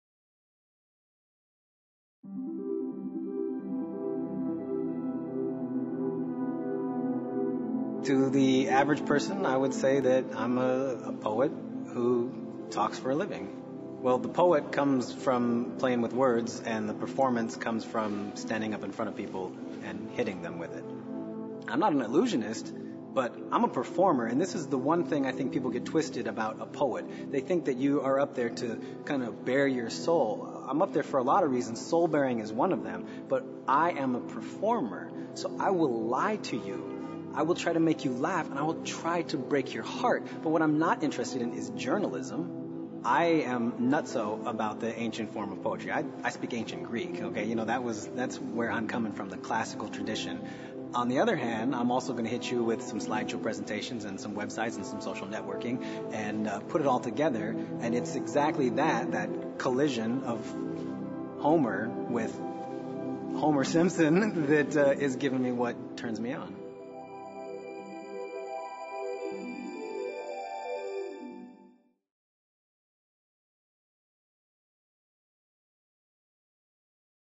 NEXT GARDE: RIVES
Storyteller, writer and slam poet Rives has appeared on the last four seasons of HBO's Def Poetry Jam and he was the 2004 National Poetry Slam champion. Offstage, Rives designs and writes pop-up books for children and interactive narratives for grown-ups.